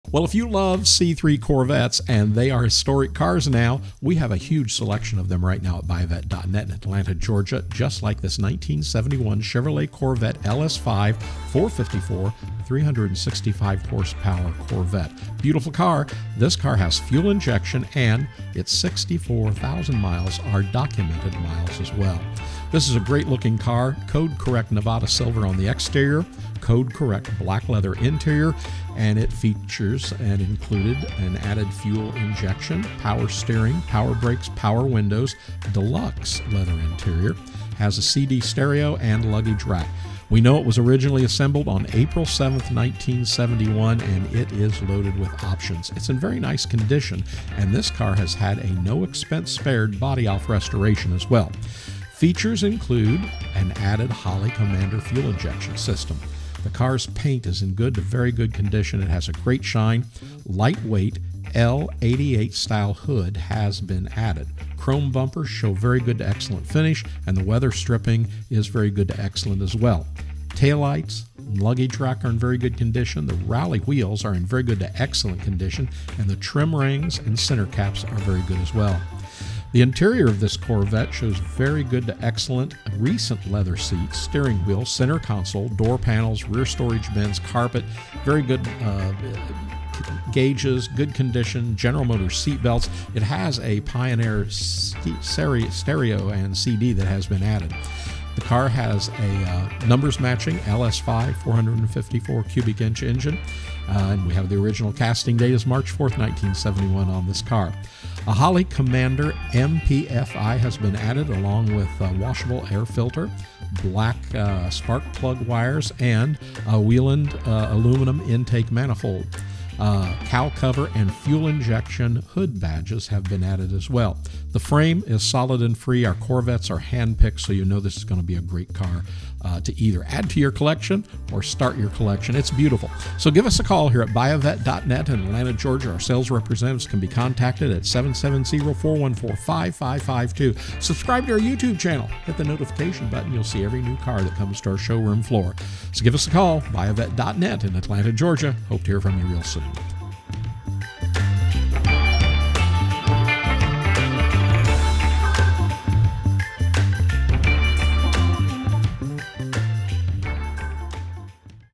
1971 Corvette LS5 454/365hp *FUEL INJECTION, 64K DOCUMENTED MILES*
1971 Corvette LS5 454/365hp *FUEL INJECTION, 64K DOCUMENTED MILES* For Sale

Code correct Nevada Silver exterior, code correct Black leather interior.

Features include added fuel injection, power steering, power brakes, power windows, deluxe (leather) interior, CD stereo, luggage rack.

Originally assembled on April 7, 1971, this 1971 Corvette LS5 is LOADED with options, in very nice condition with a NO EXPENSE SPARED BODY-OFF RESTORATION, and features an added Holley Commander fuel injection system. This car's paint is good-vg, with a great shine. Lightweight L88 style hood has been added. Chrome bumpers show a vg-exc finish, and weatherstripping is vg-exc as well. Taillights and luggage rack are vg, rally wheels are vg-exc, and trim rings and center caps are vg as well. Radial raised white letter tires show an average of 10/32nds tread depth remaining. The interior of this 1971 Corvette LS5 shows vg-exc recent leather seats, steering wheel, center console, door panels, rear storage bins, and carpet, vg gauges, and good condition GM seatbelts. A Pioneer CD stereo has been added. The numbers matching LS5 454ci engine in this car shows original casting date March 4, 1971, and final assembly date March 30, 1971. Correct air cleaner, valve covers, radiator hoses, and ignition shielding are in place. Holley Commander MPFI has been added, along with a washable air filter, Black spark plug wires, and a Weiand aluminum intake manifold. Cowl cover and "Fuel Injection" hood badges have been added as well. Frame is solid and rust-free. Included with this car will be a binder of extensive receipts and invoices totaling nearly $75,000, and dating back to 2003, as well as a reproduction window sticker.

64K Miles.